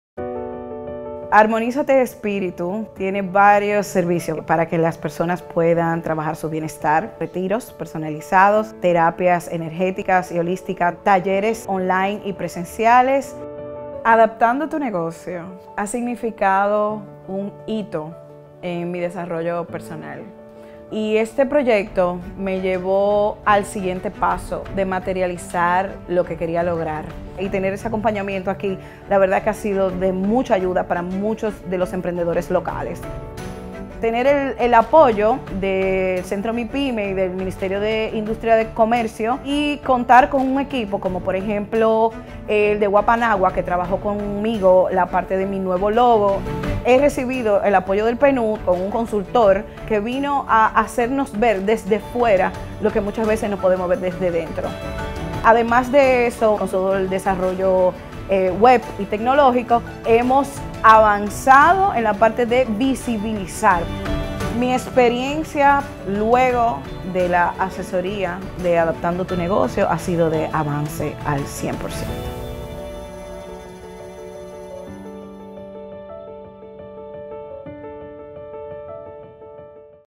AdaptandoTuNegocio: Armonízate Espíritu
Con Adaptando tu Negocio, Rosa Amarella aprendió a enfocar sus ideas de negocio para transformarles en bienestar para ella, su familia y sus clientes.

Adaptando Tu Negocio es una metodología diseñada por PNUD y que fue testeada por primera vez en la República Dominicana, único país en toda la región de América Latina y el Caribe seleccionado, y el tercero a nivel mundial junto a Albania y Barbados.

El programa fue implementado por el Programa de las Naciones Unidas para el Desarrollo (PNUD) con el apoyo del Ministerio de Industria, Comercio y Mipymes (MICM), el Ministerio de Turismo, el Ayuntamiento de Las Galeras, el Consejo de Turismo Sostenible de Las Galeras (CONTUGAL) y el Centro Mipymes UAPA Nagua, que tiene el objetivo de apoyar a la recuperación y fomento de la resiliencia de las mipymes en esta zona turística.